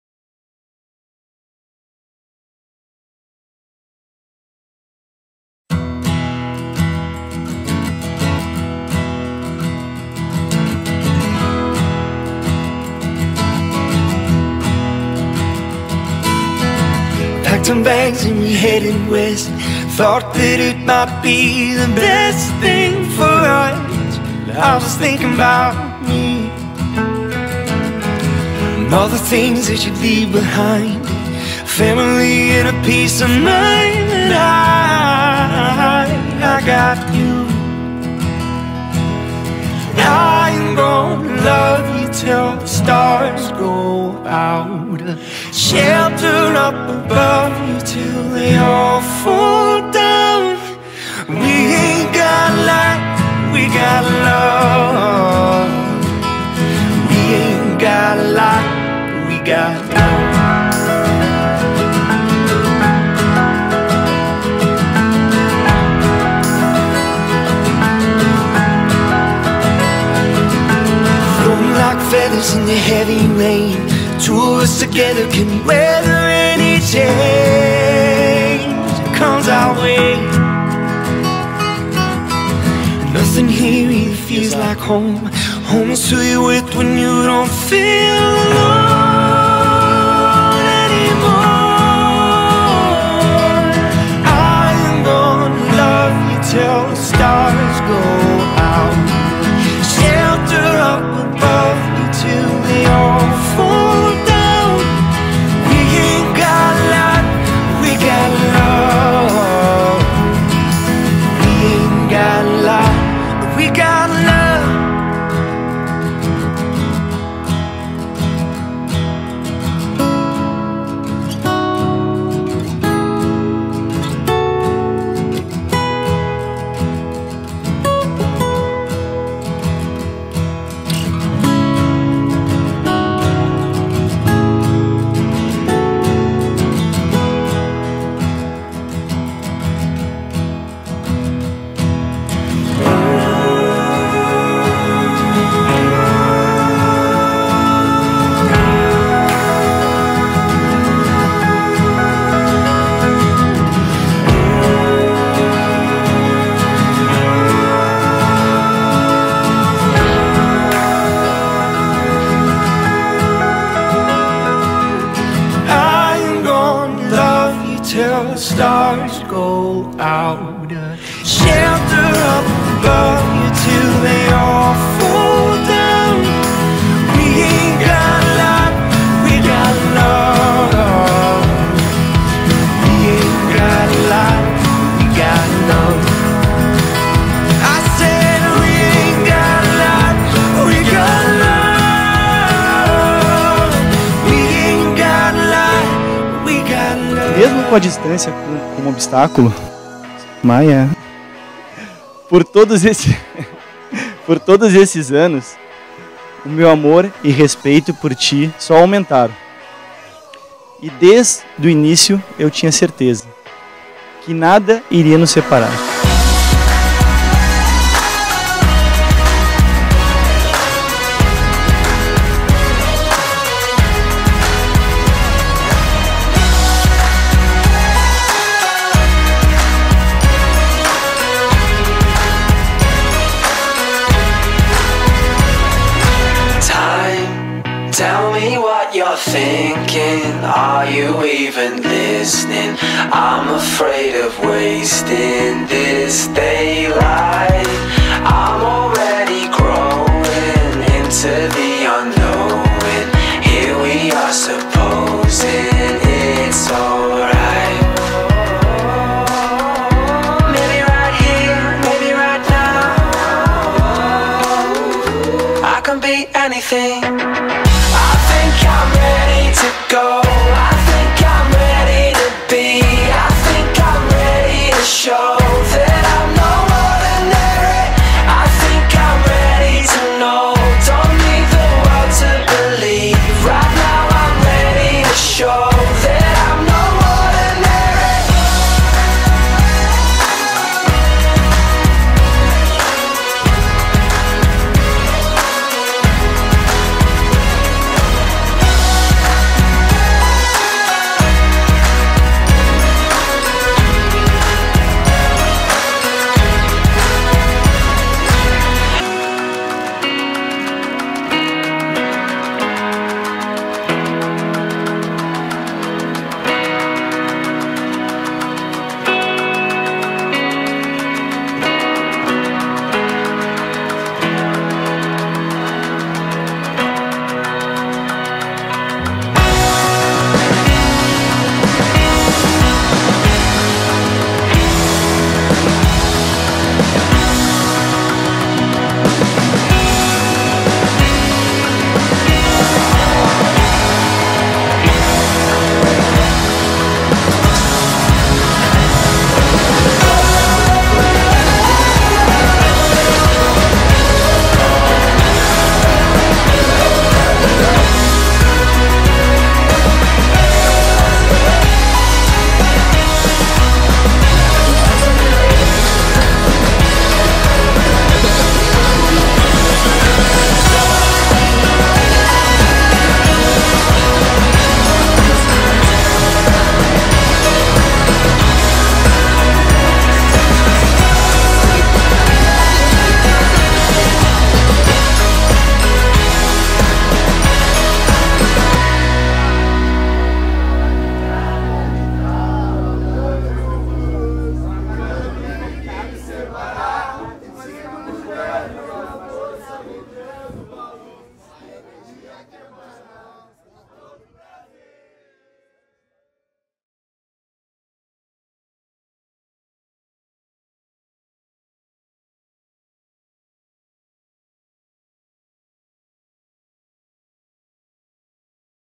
Casamento LINDO na Praia do Rosa! Dani + Lucas - GuilhermeCoelhoTV
Captado pelas lentes de Guilherme Coelho. Casamento feito com apenas 1 cinegrafista e 100% dos takes na mão